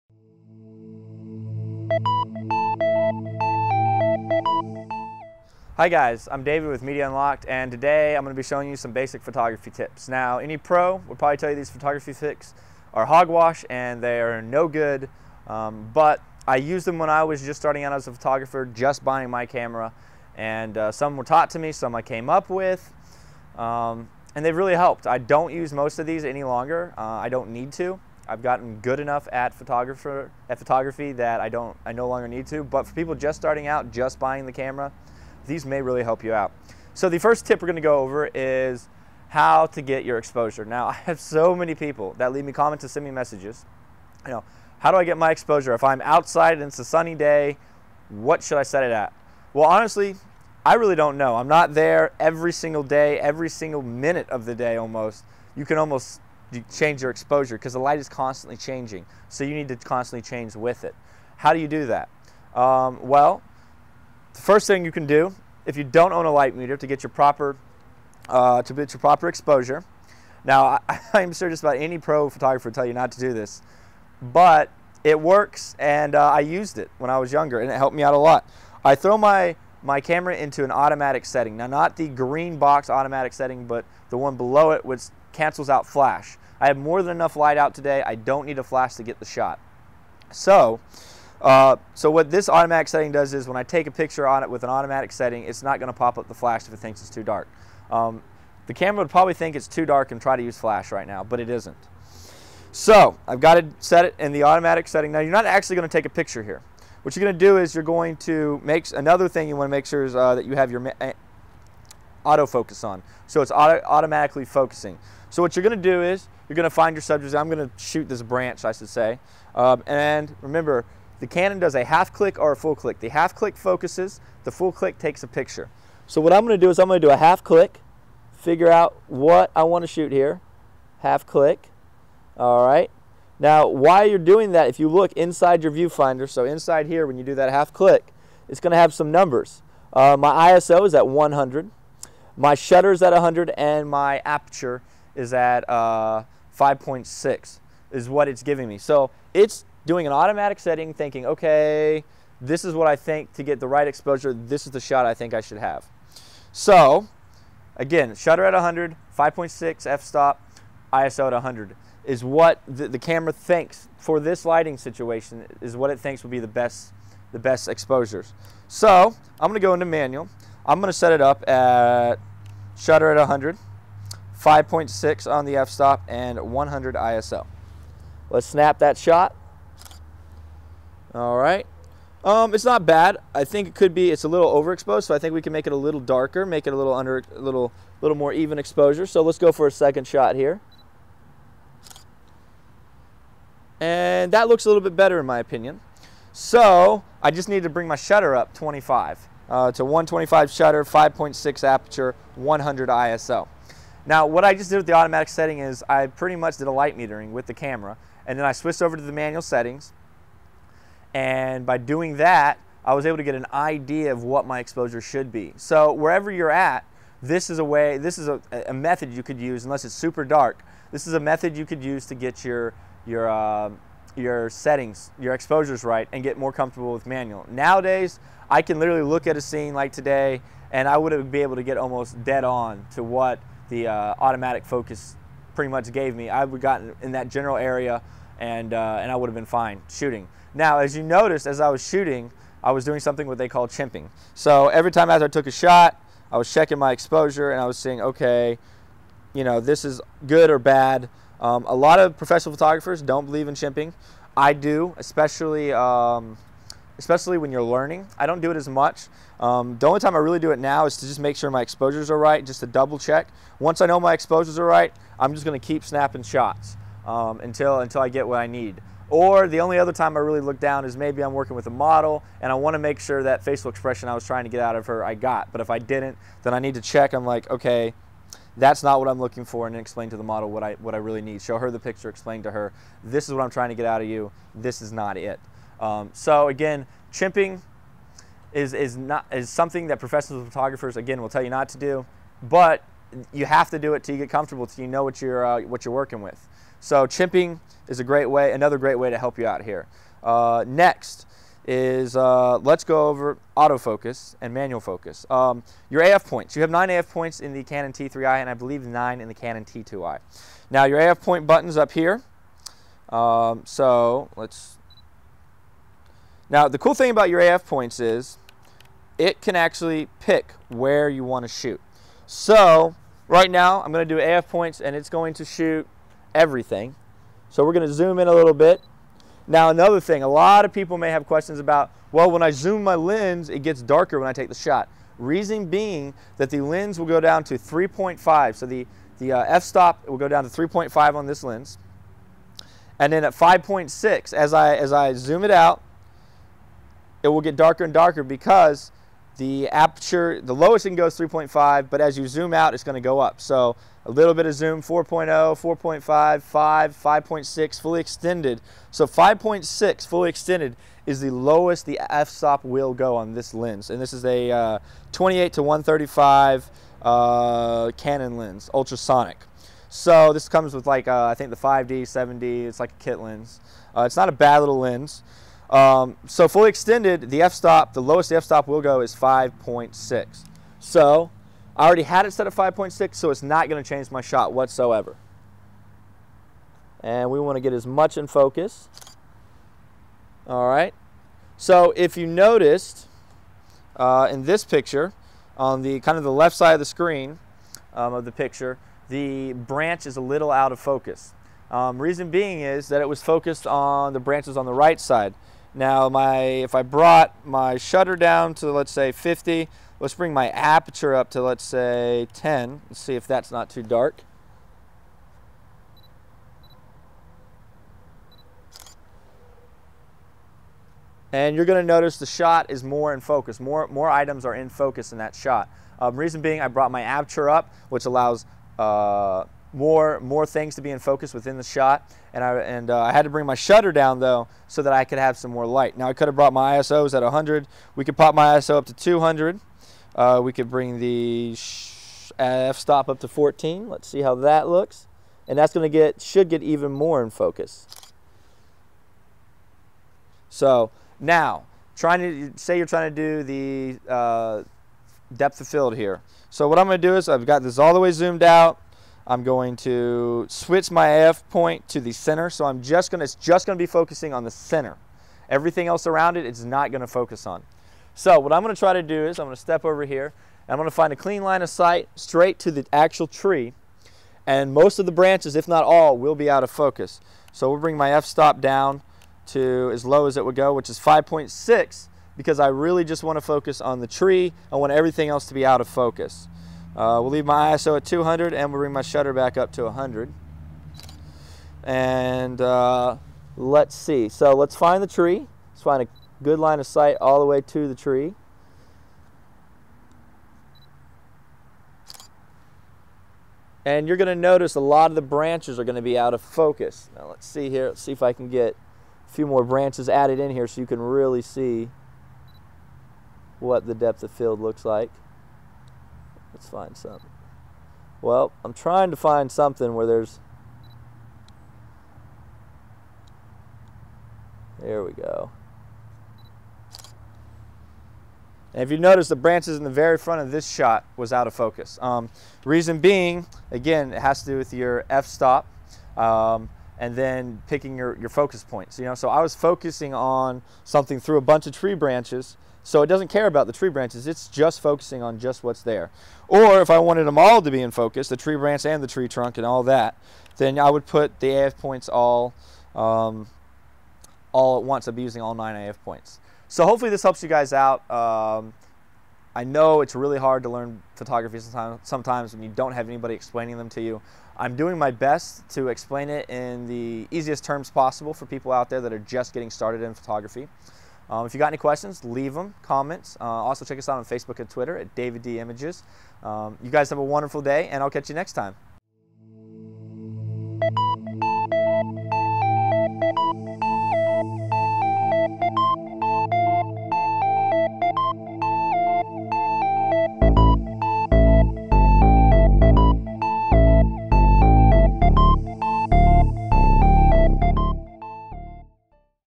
Photography Basic 101 Tips
In the video I go over how to get your exposure right for different shots in different settings. Why chimping is good if you are just starting out as a photographer. How to get a shallow depth of field or a very focused shot. I also go over auto focus points as well.

SKYPE
DAVIDDIMAGE